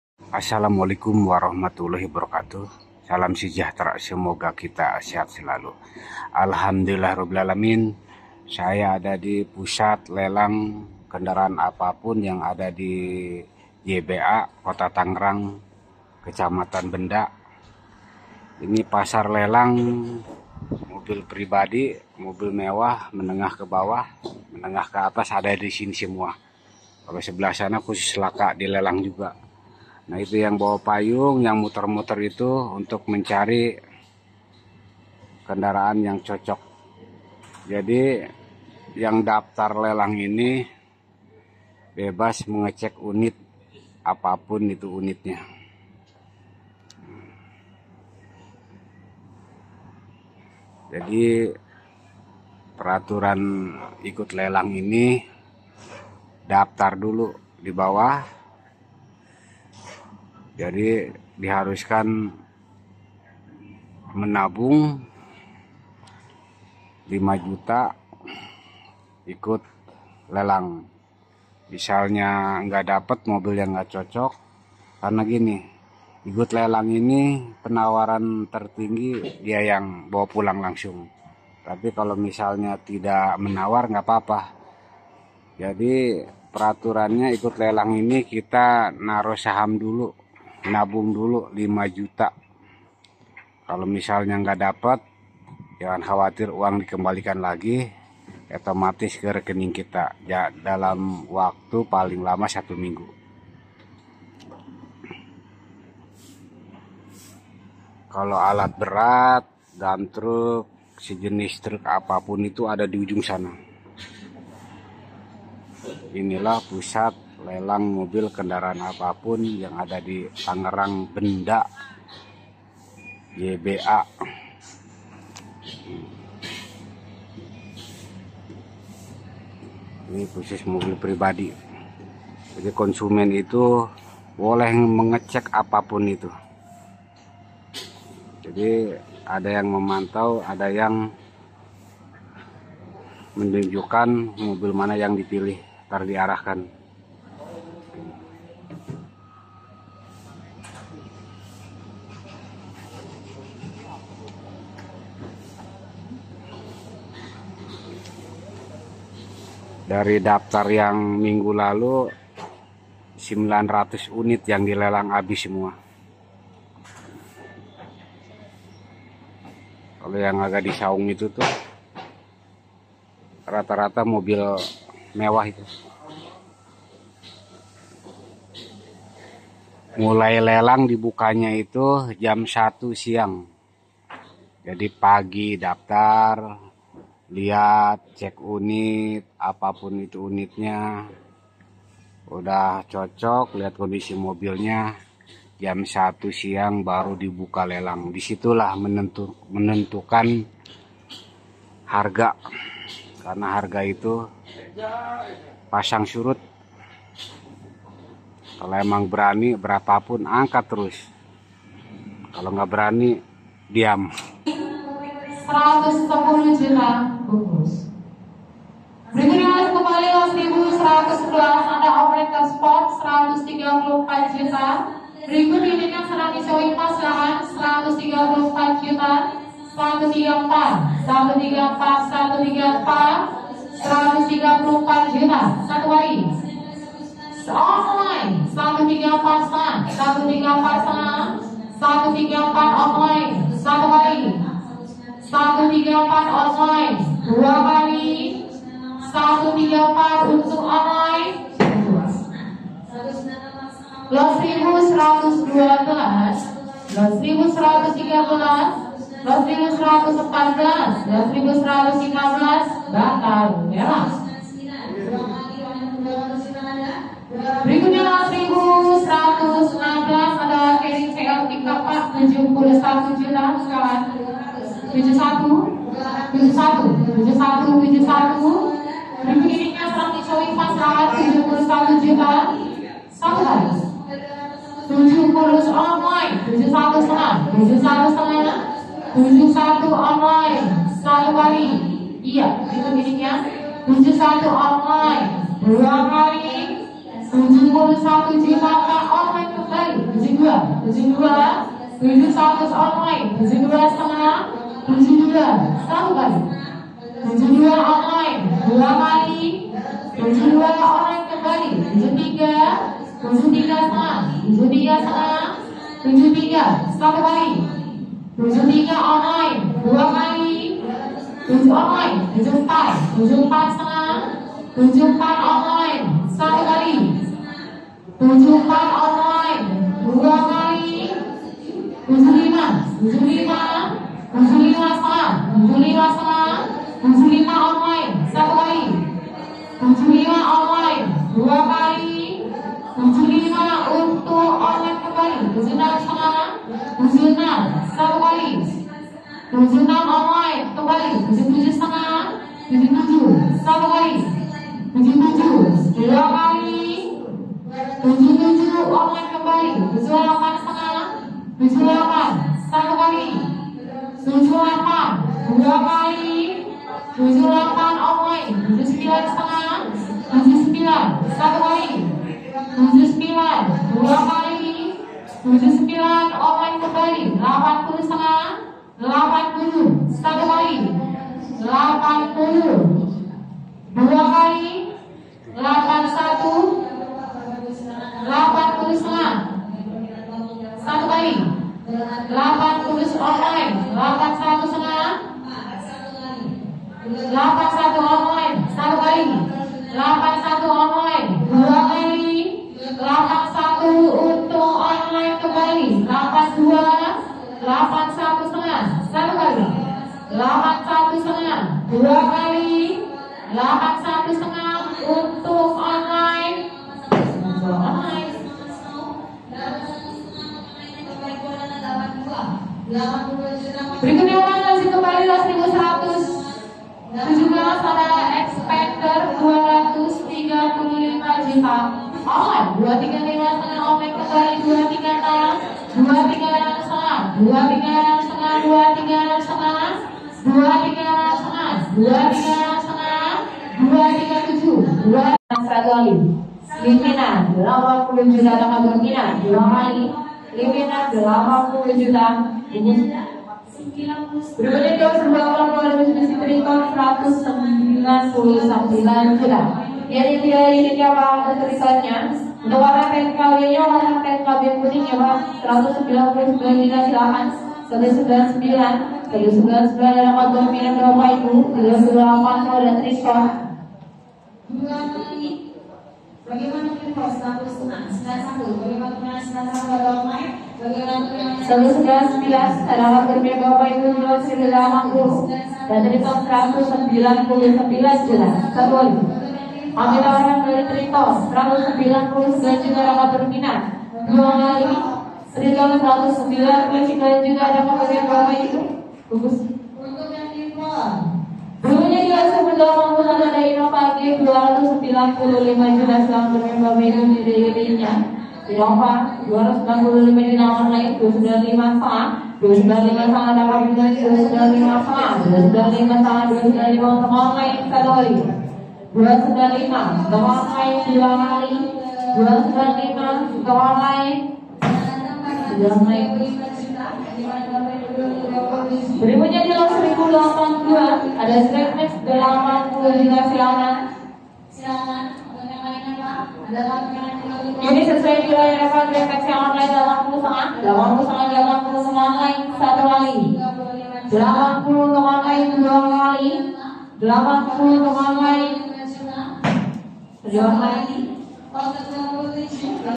ATURAN IKUT LELANG MOBIL JBA JAKARTA RAYA MOBIL COCOK LANGSUNG BAWA PULANG
jba jakarta raya pusat mobil lelang mobil cocok langsung bawa pulang yang ada di tangerang kota jalan husein sastra negara nusa indah 100 meter pintu masuk nyah dari kantor kecamatan benda tangerang